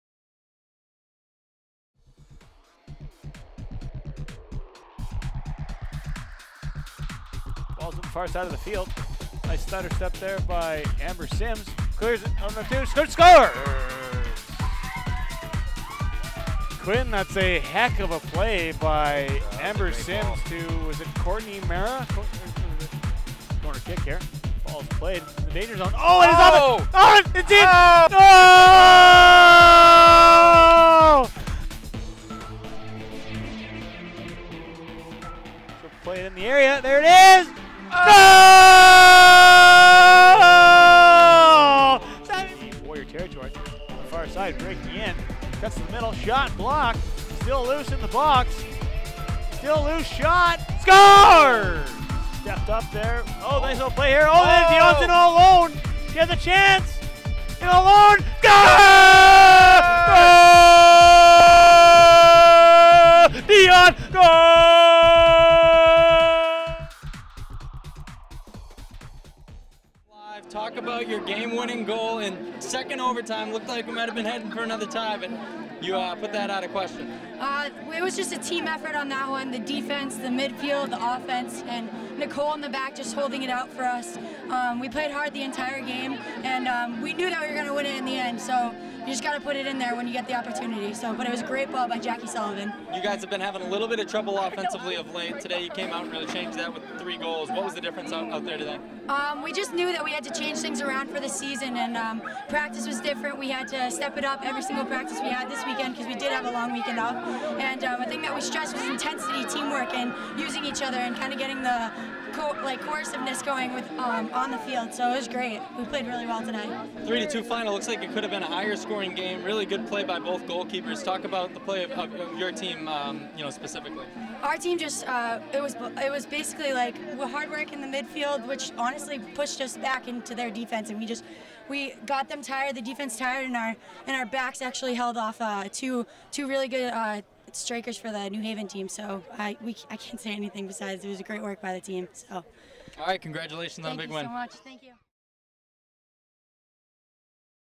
Women's Soccer (10/5/13) Merrimack - 3, New Haven - 2 (2 OT)
Merrimack College took down New Haven in double-overtime as Danielle Dion came away with the game winner. Exclusive interview with Dion following the highlights.